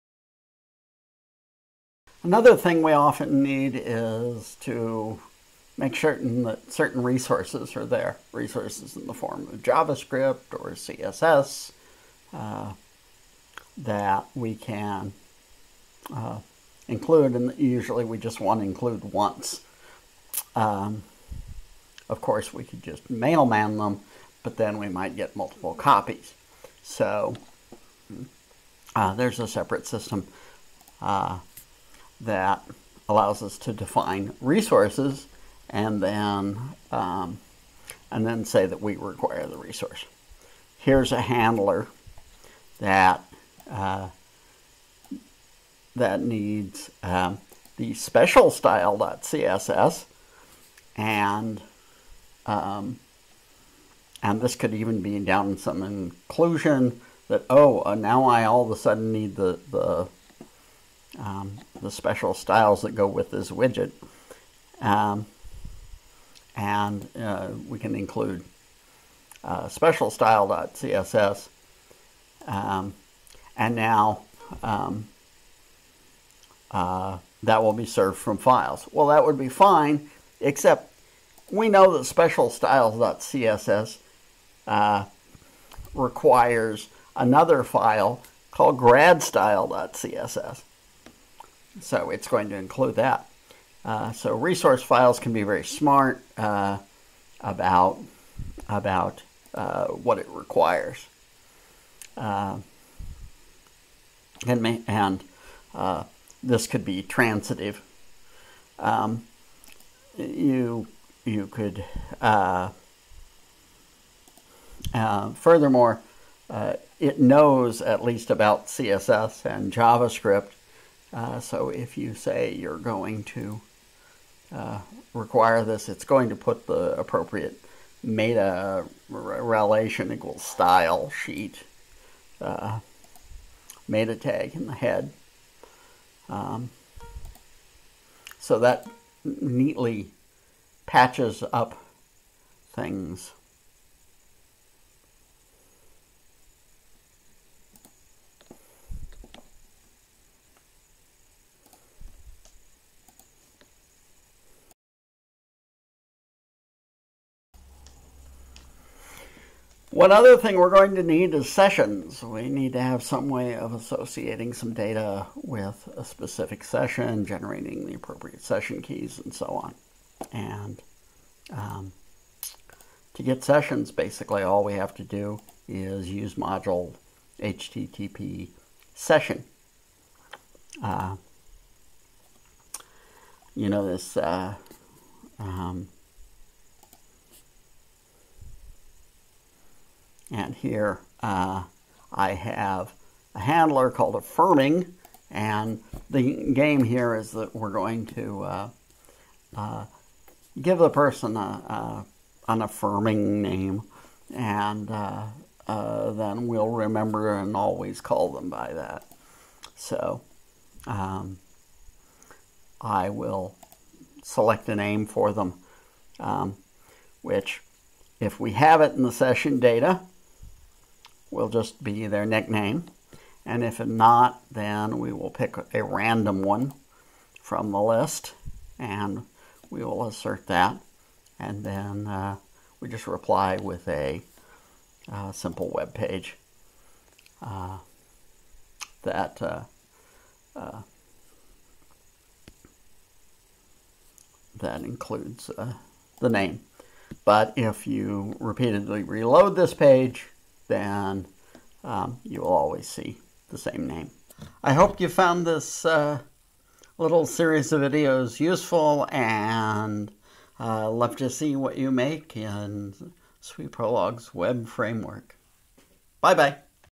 Prolog Web Framework - Episode 10 - Resources, Sessions
Episode 10 of Prolog Web Framework
Based on the workshop I gave in 2013 at Strange Loop